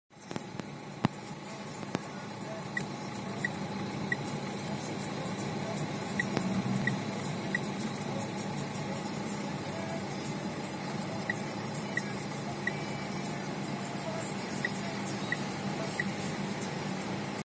Меган 3 "вежливые" поворотники )))
Меган 3 "вежливые" поворотники ))) Делал renolink v1.87 .
В блоке UCH ( bcm ) находим строку Flasher заходим в Flasher configuration и в правом окне выбираем TPULSE_CF прописываем true в замен false .